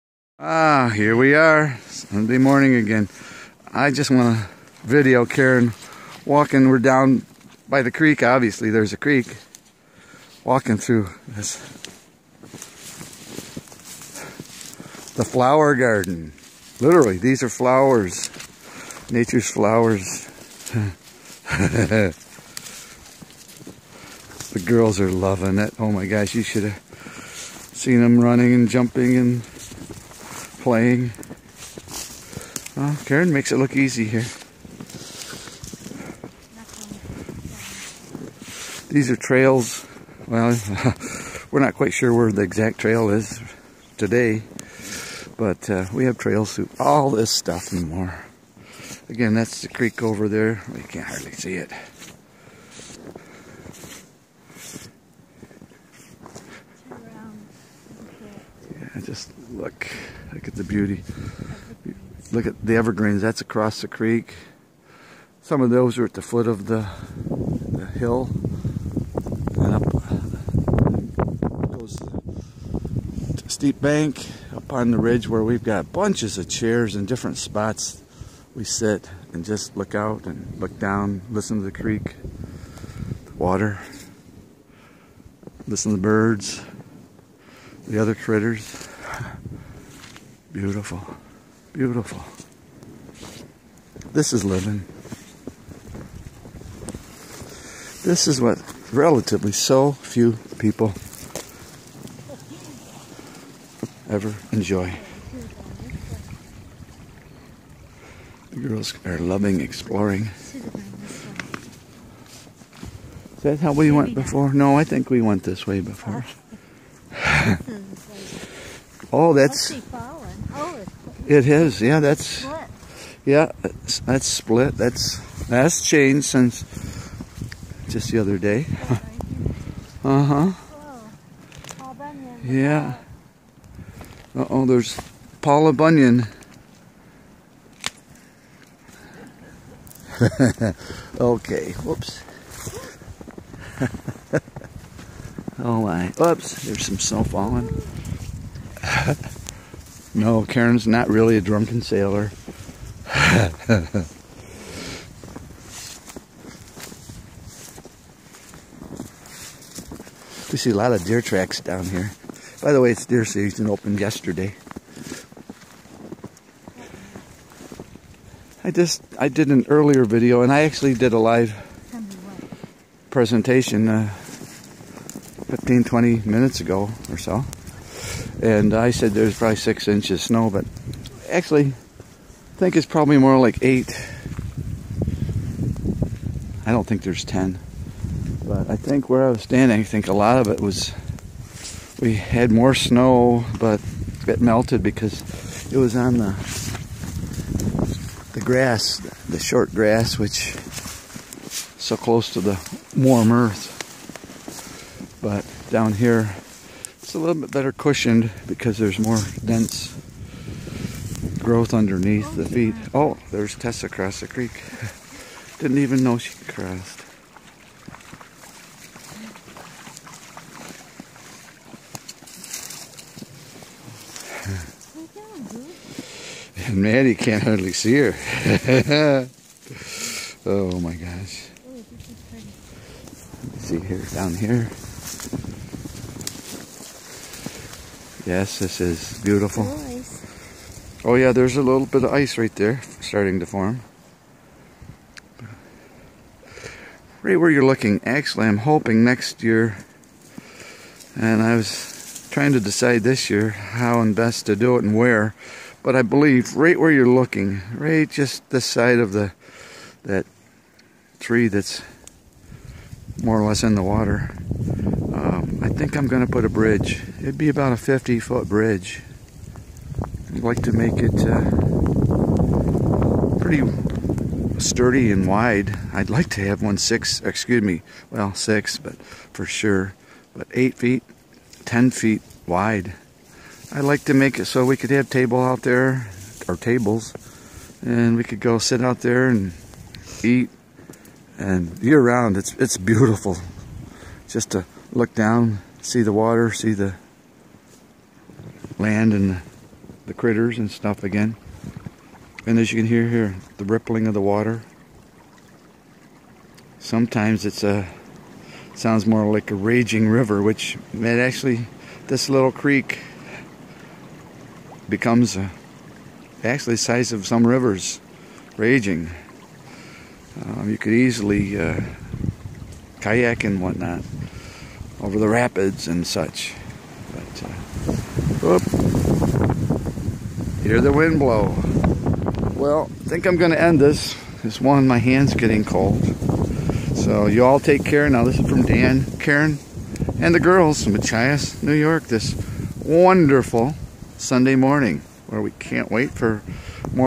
First Season Stroll Through Our Flower Garden Along Our Creek -20161120 1104271
Beauty ; Not "The Least" the magic of the creek flowing, and the snow falling, the dogs playing and us, well "Just Loving It"!